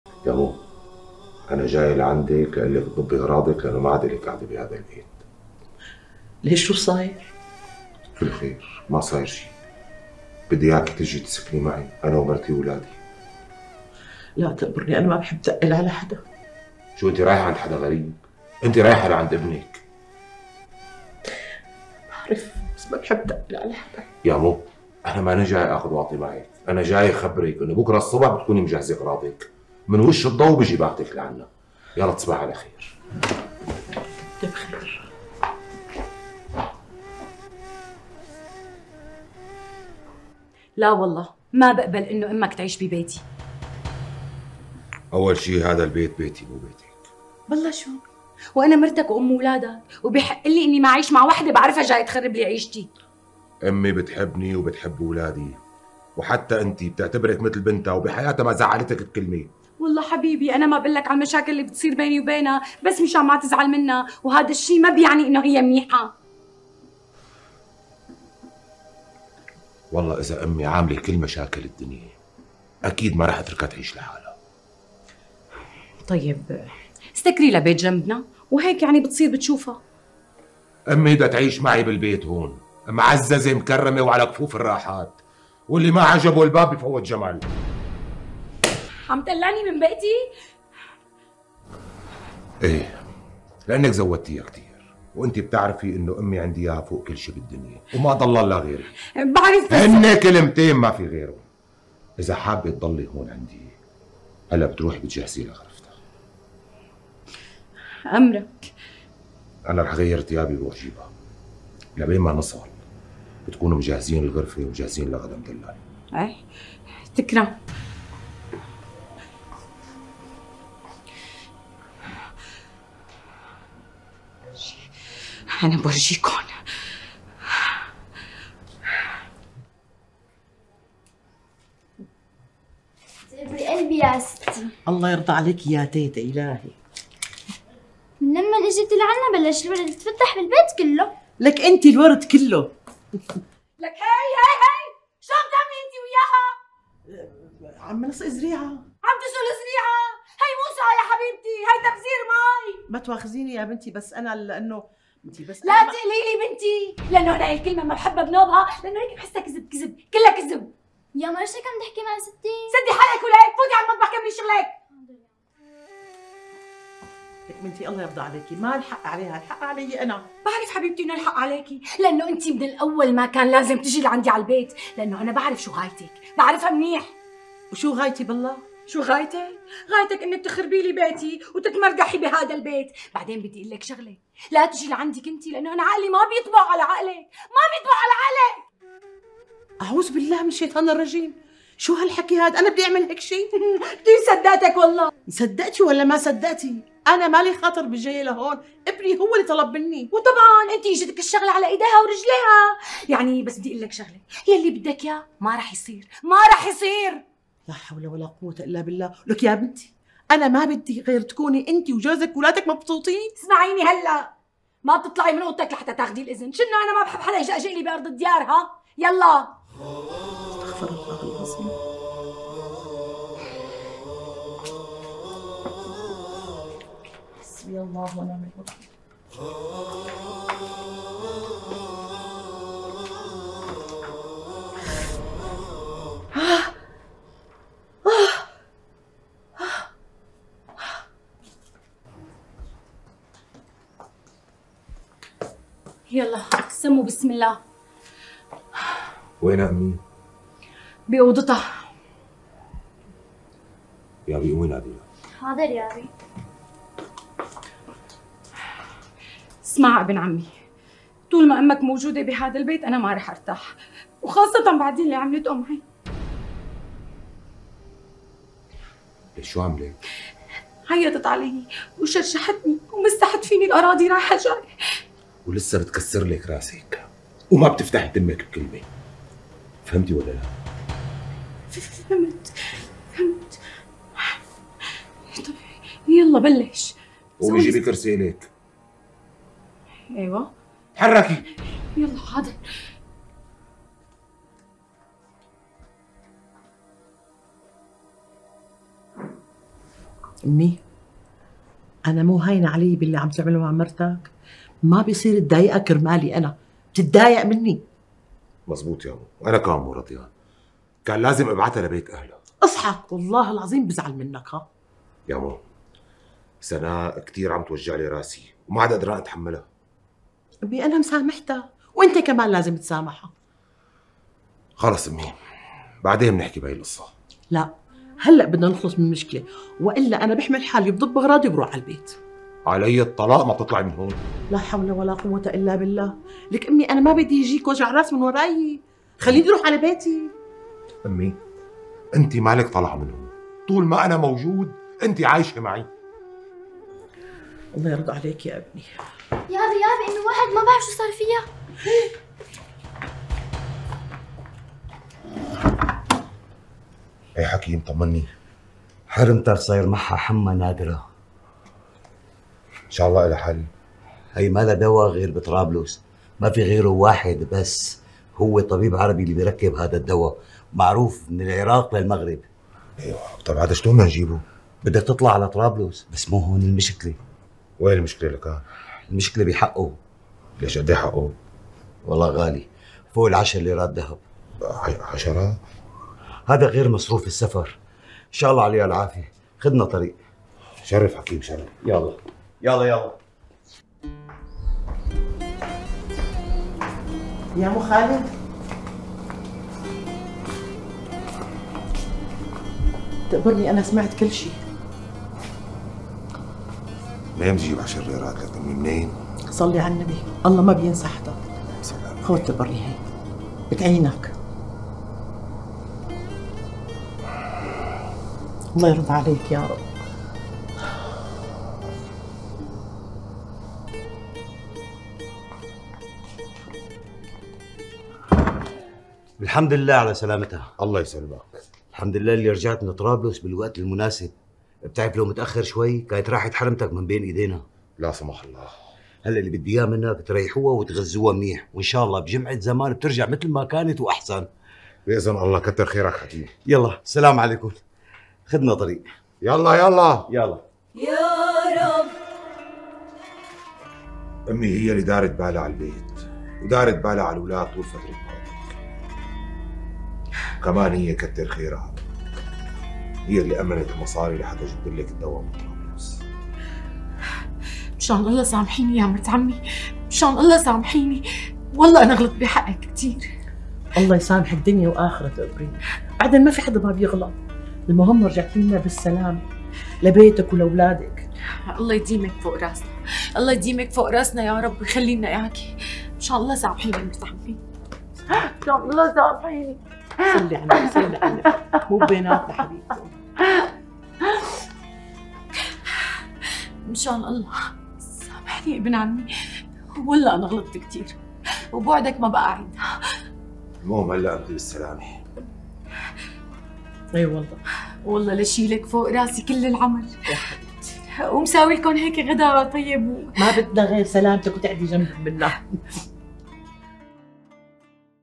رفضت تعيش حماتها معها بنفس البيت وبدها زوجها يرمي أمه بالشارع بس بالأخير الله ابتلاها بمرض خطير!💔
رفضت تعيش حماتها معها بنفس البيت وبدها زوجها يرمي أمه بالشارع مشان ترتاح وحقدها ينتصر بس بالأخير الله ابتلاها بمرض خطير!💔
من روايات
رواية "أم خالد والكنة"